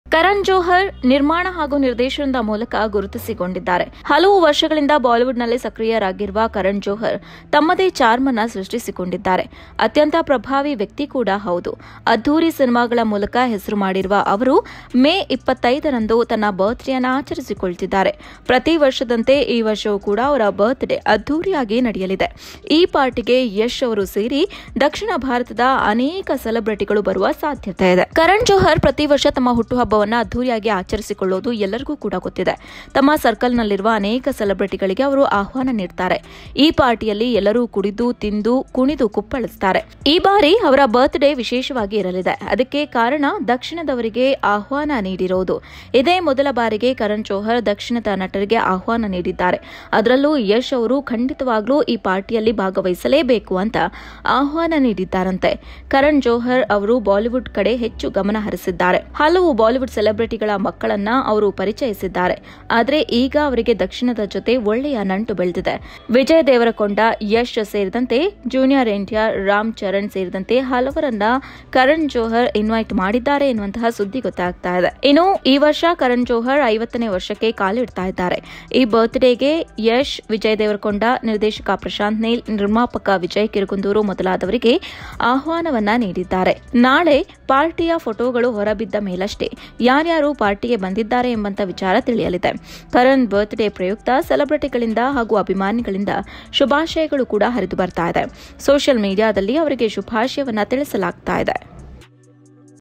ಸೌತ್‌ನ ಯಾವೆಲ್ಲ ನಟರು ಇರಲಿದ್ದಾರೆ ಕರಣ್ ಪಾರ್ಟಿಯಲ್ಲಿ?
Dharma Production owner Karan Johar likely to invite Yash and other south stars to his birthday party

ಇಂದು(ಮೇ 25)ಬಾಲಿವುಡ್ ಕರಣ್ ಜೋಹರ್ ಹುಟ್ಟುಹಬ್ಬ. ಈ ಬರ್ತ್‌ಡೇ ಪಾರ್ಟಿಗೆ ಕರಣ್ ಜೋಹರ್ ಸೌತ್‌ನ ಹಲವಾರು ನಟರನ್ನು ಆಹ್ವಾನಿಸಿದ್ದಾರೆ. ಅಲ್ಲದೇ ಯಶ್ ಕೂಡ ಬರ್ತ್‌ಡೇಗೆ ವಿಶೇಷ ಆಹ್ವಾನಿತರು.